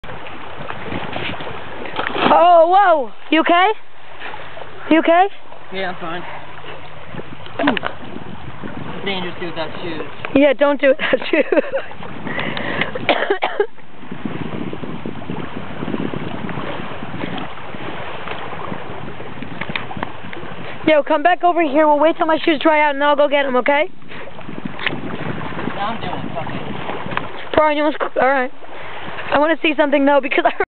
Bouldering Fall
My funny fall on my face into the water while bouldering